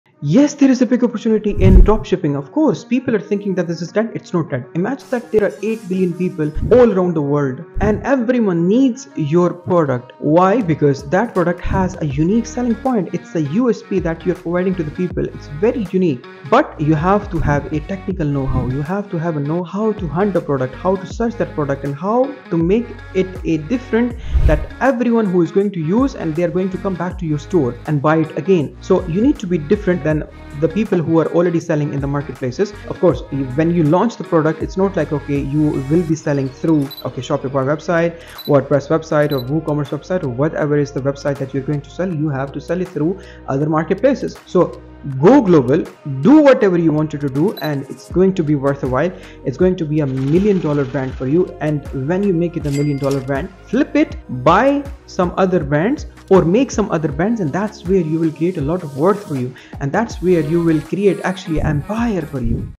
Drop shipping | Nouman Saeed
Find the secrets of dropshipping success and ride the wave of eCommerce profitability. 🛍️💡 Learn how to spot winning products, build a lucrative online business, and create a lifestyle of freedom. 💻💰 Don't miss out on this game-changing opportunity!

𝐅𝐨𝐥𝐥𝐨𝐰 𝐟𝐨𝐫 𝐦𝐨𝐫𝐞 𝐬𝐮𝐜𝐡 𝐬𝐧𝐞𝐚𝐤 𝐩𝐞𝐞𝐤 𝐞𝐱𝐜𝐥𝐮𝐬𝐢𝐯𝐞 𝐜𝐨𝐧𝐭𝐞𝐧𝐭!